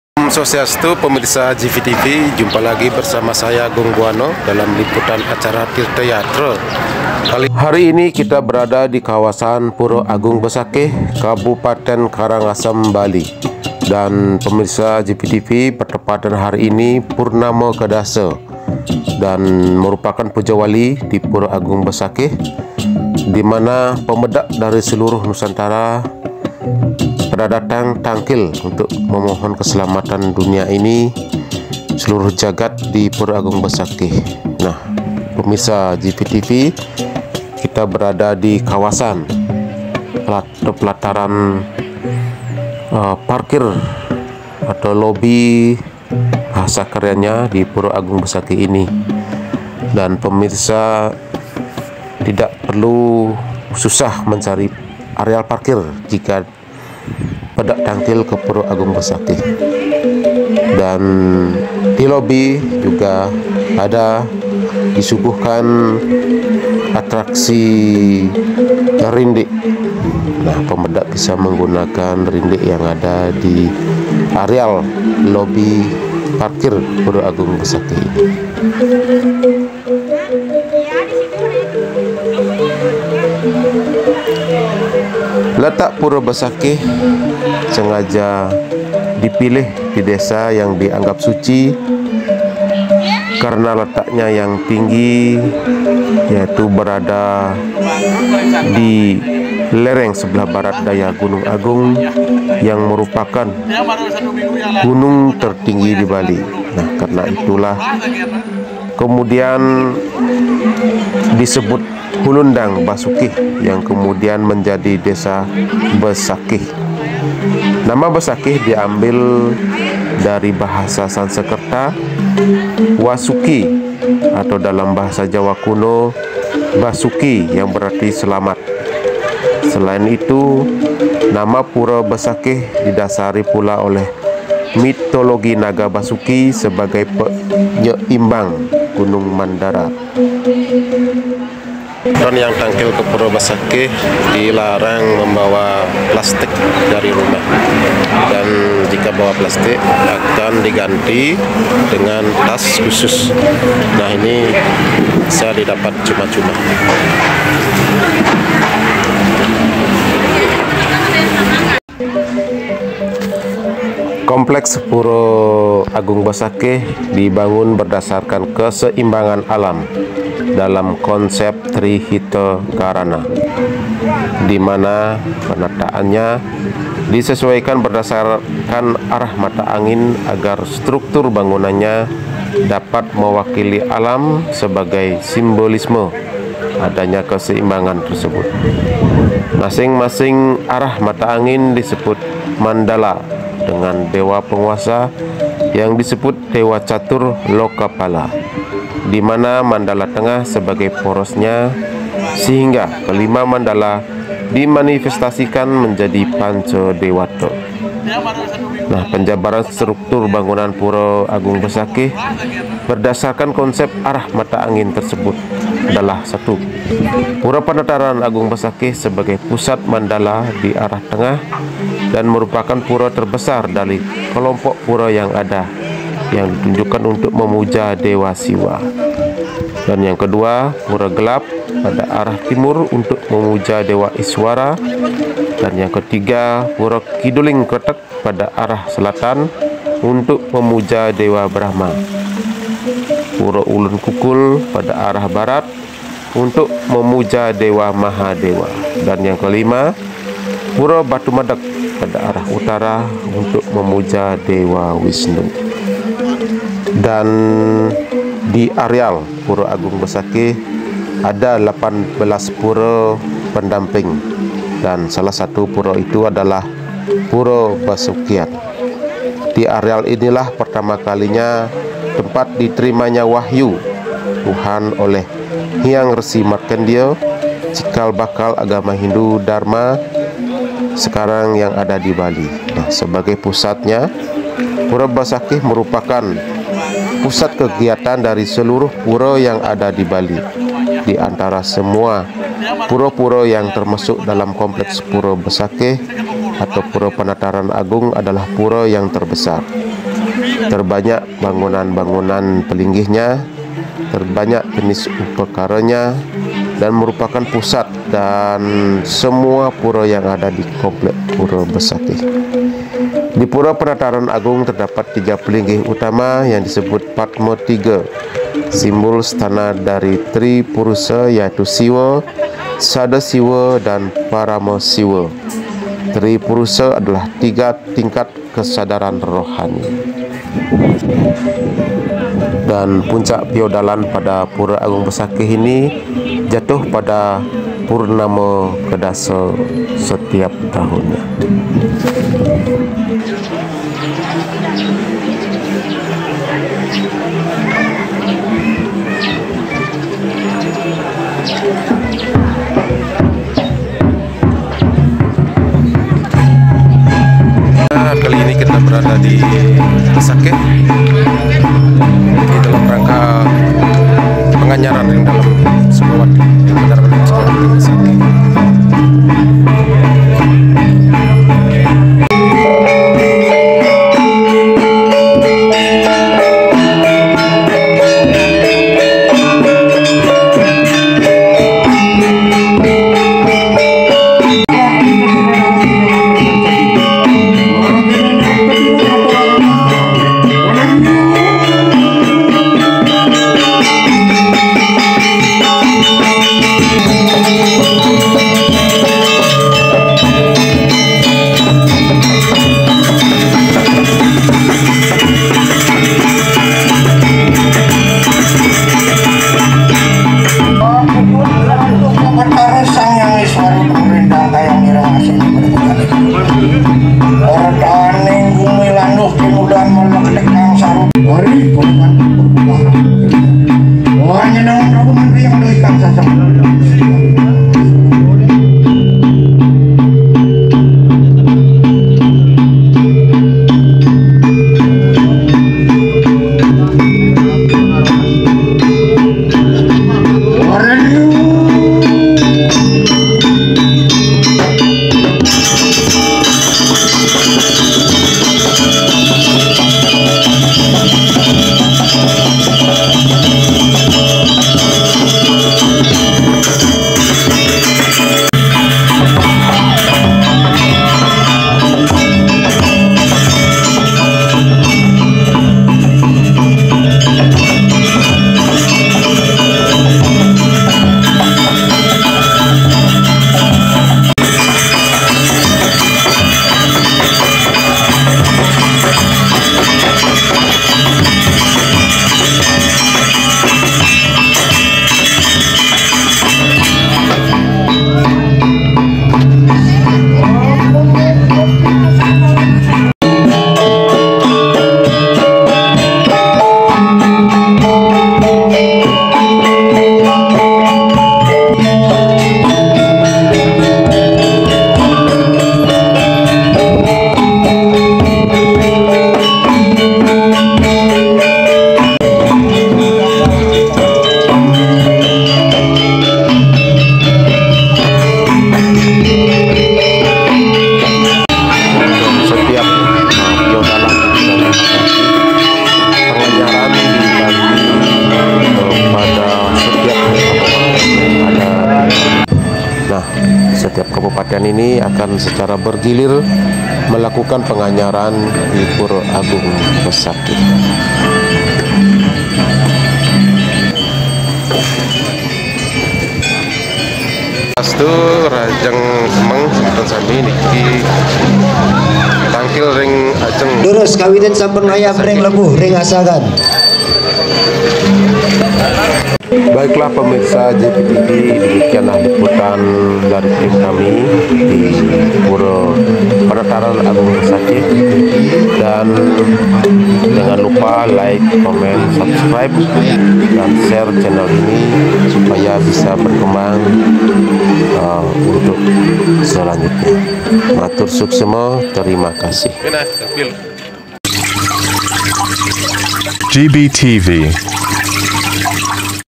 PURA PENATARAN AGUNG BESAKIH
Pura Besakih adalah sebuah komplek Pura yang terletak di Desa Besakih, Kecamatan Rendang, Kabupaten Karangasem, Bali, Indonesia. Komplek Pura Besakih terdiri dari 1 Pura Pusat dan 18 Pura Pendamping. Di Pura Basukian, di areal inilah pertama kalinya tempat diterimanya wahyu Tuhan oleh Hyang Rsi Markendya, cikal bakal Agama Hindu Dharma sekarang di Bali, sebagai pusatnya. Pura Besakih merupakan pusat kegiatan dari seluruh Pura yang ada di Bali. Di antara semua pura-pura yang termasuk dalam kompleks Pura Besakih, Pura Penataran Agung adalah Pura yang terbesar, terbanyak bangunan-bangunan pelinggihnya, terbanyak jenis upakaranya dan merupakan pusat dan semua Pura yang ada di komplek Pura Besakih. Di Pura Penataran Agung terdapat 3 pelinggih utama yang disebut Padma Tiga simbol stana dari Tri Purusha yaitu Siwa, Sada Siwa dan Parama Siwa. Tri Purusha adalah tiga tingkat kesadaran rohani.